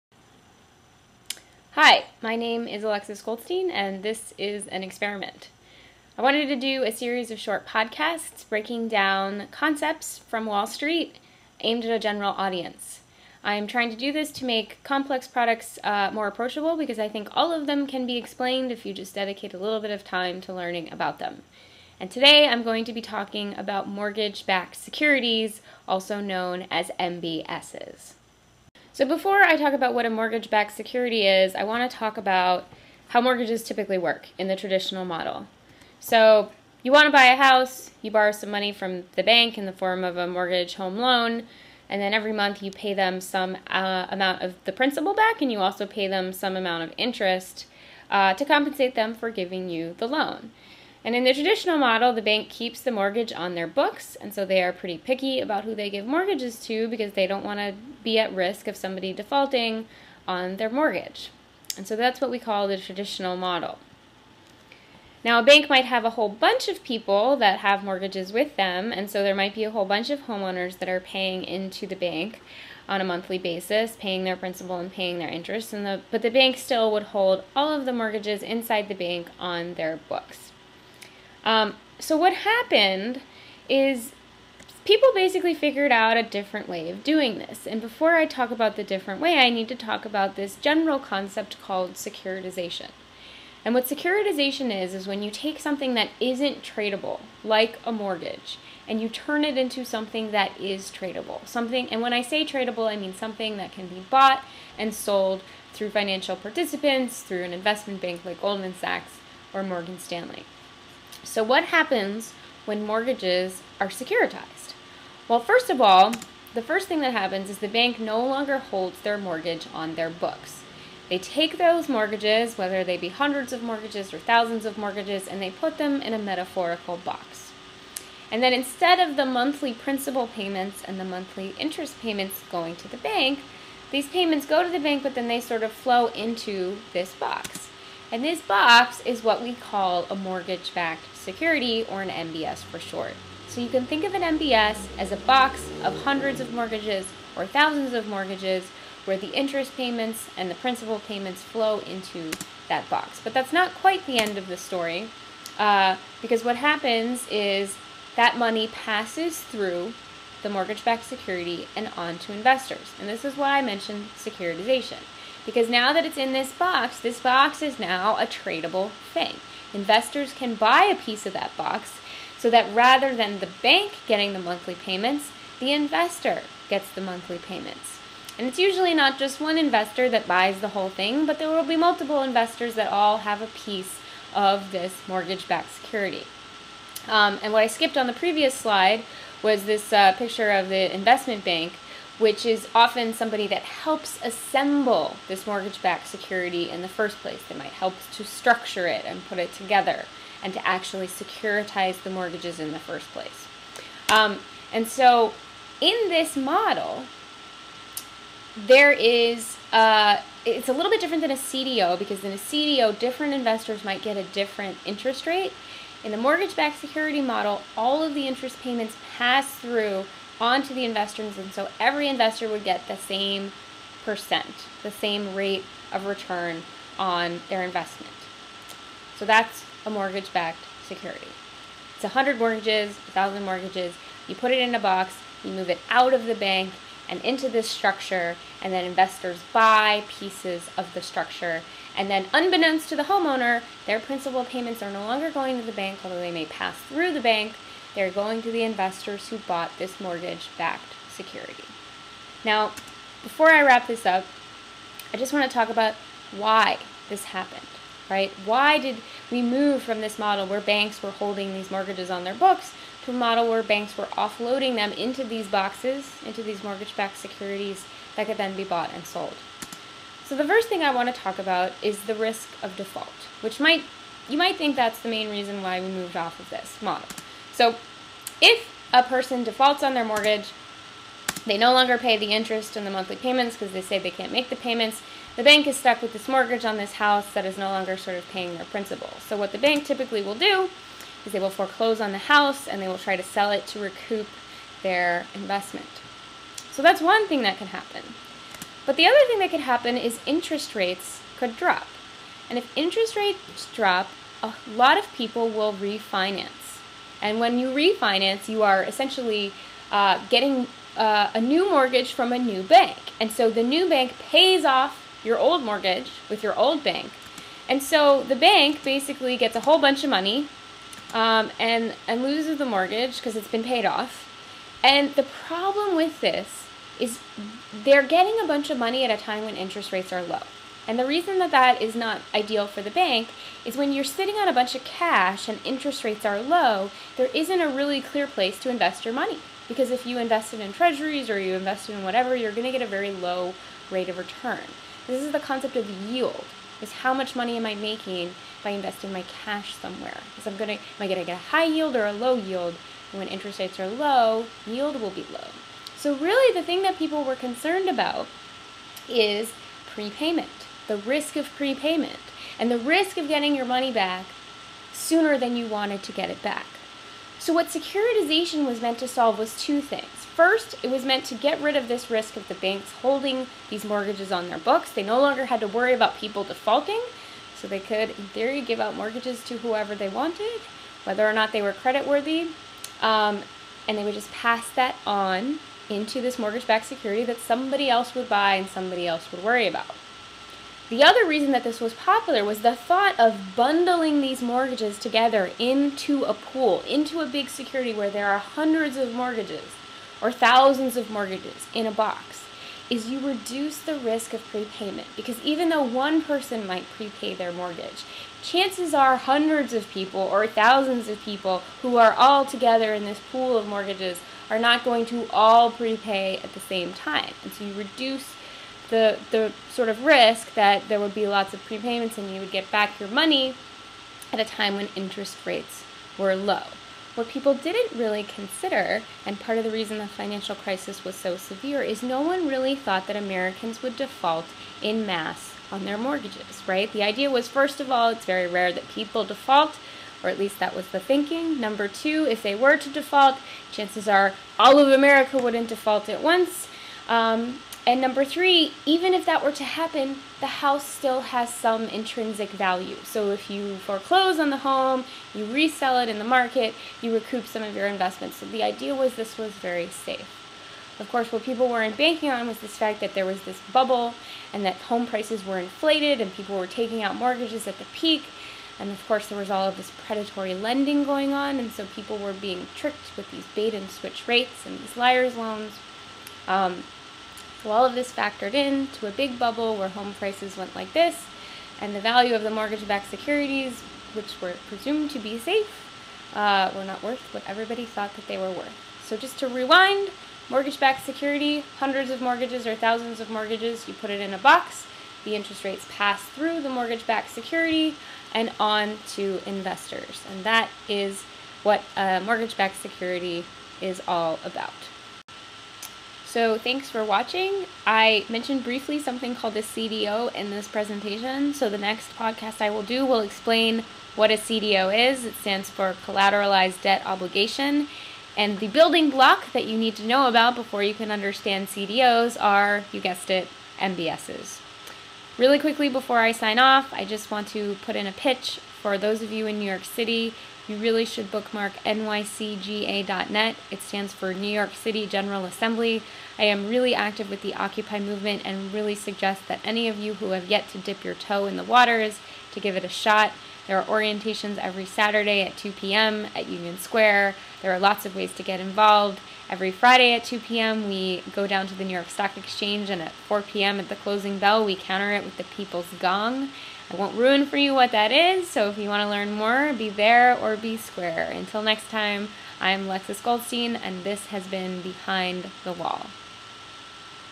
What is a Mortgage Backed Security (MBS)?
An overview of the financial product Mortgage Backed Security (MBS). Describes the traditional model of mortgage lending, and how that changed with the introduction of the MBS. I also provide a brief overview of what Securitization is, and how MBSes played into the 2008 crash.

This is the first in a series of explainers on basic financial products.